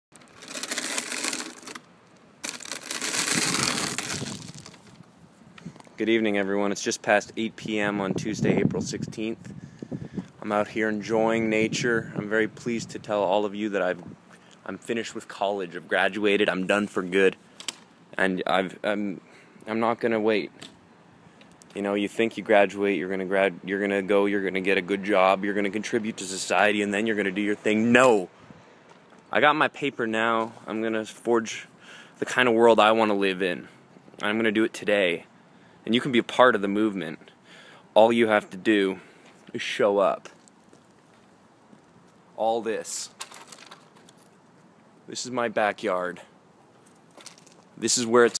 Occupy Mind-Street
I just graduated college and I'm starting a movement to change the world. Ran out of space on my iPhone before the video was finished, but you get the idea.

Just show up. It's all it takes.